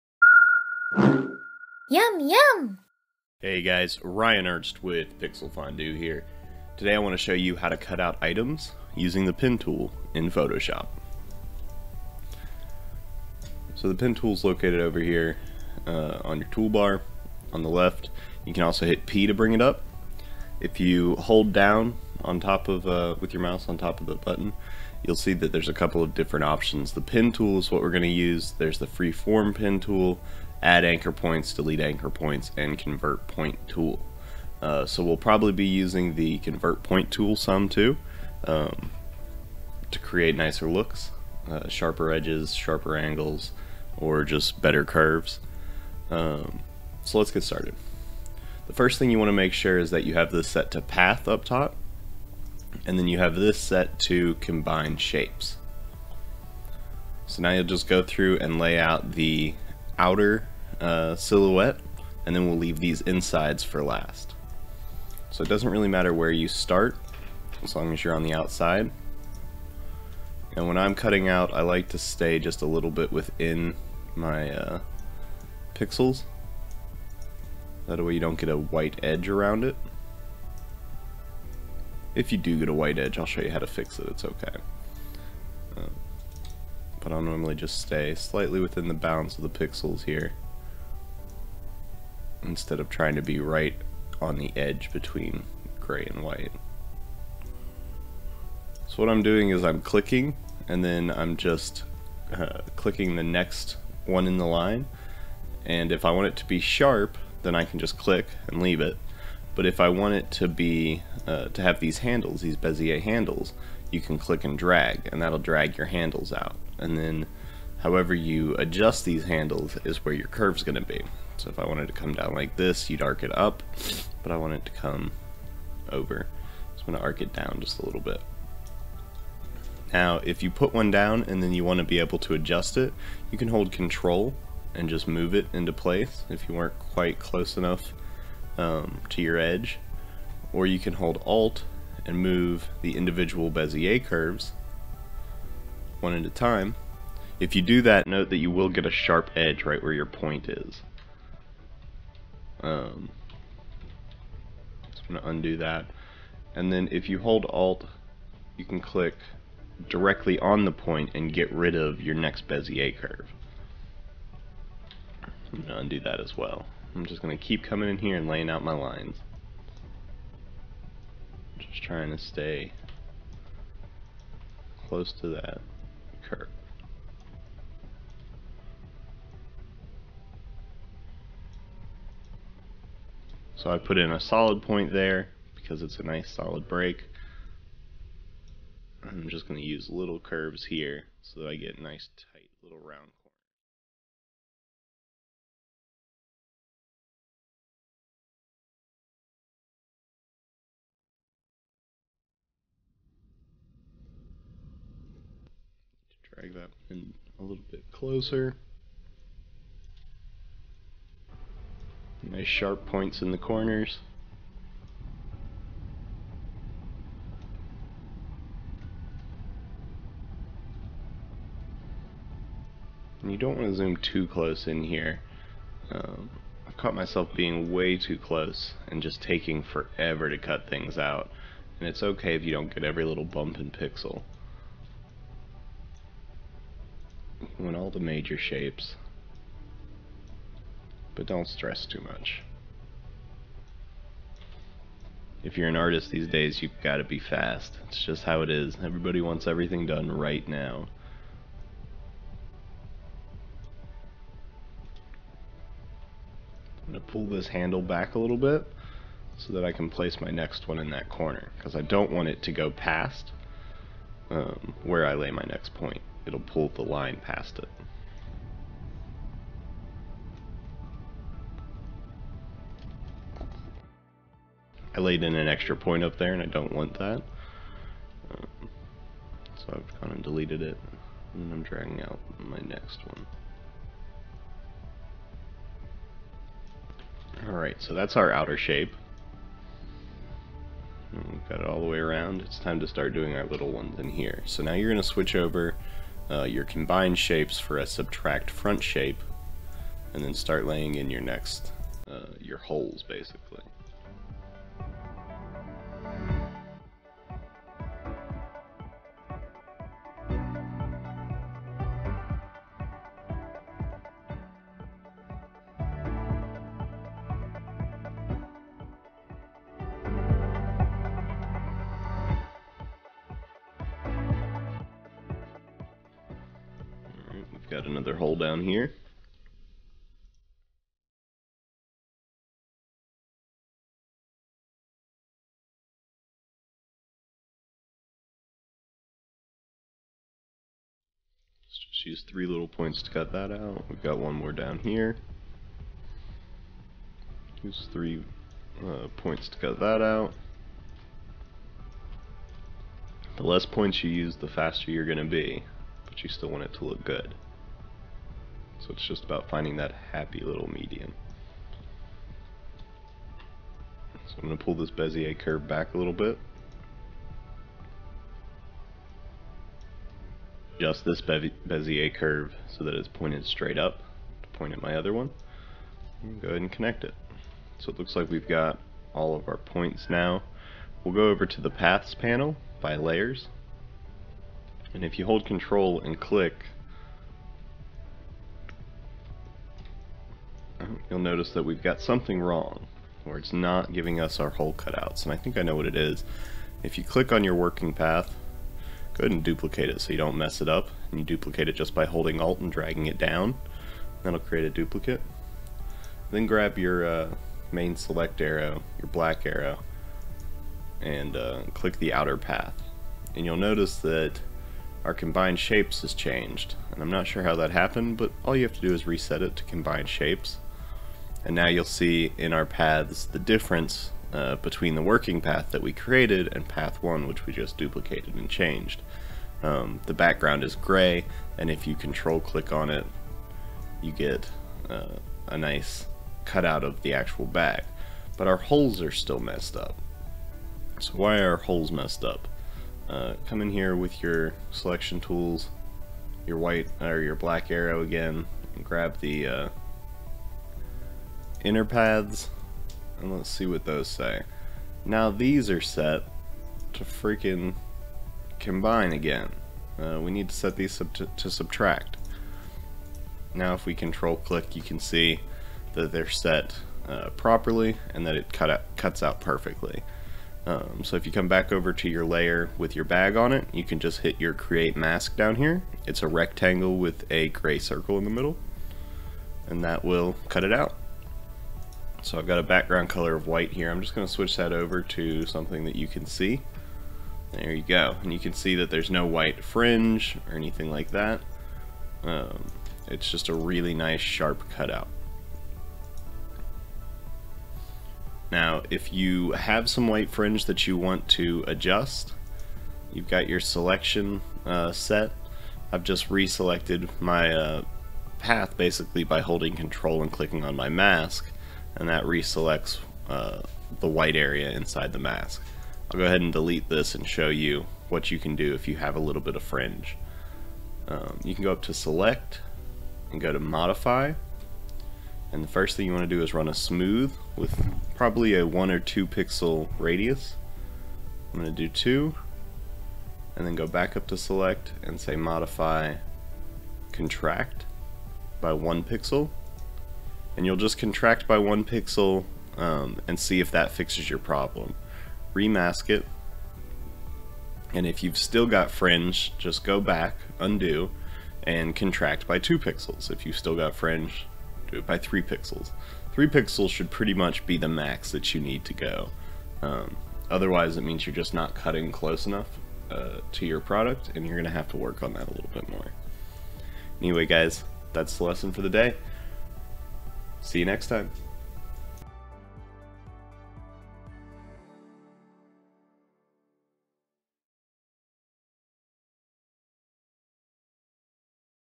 Cut Out Items in Photoshop with the Pen Tool
Cutting out product images is a big role in a lot of graphics departments.  Cutouts are used in everything from print design to website shots.  You need clean cut lines and you need to keep your files editable.  Using the pen tool is, in my opinion, the best way to handle this in most cases.   This is also a solid way to get better at drawing vectors with the pen tools in Photoshop and Illustrator.  Using the pen tool for your cutouts will teach you to be fast with the pen tool and will help you add subtlety to your lines in the future.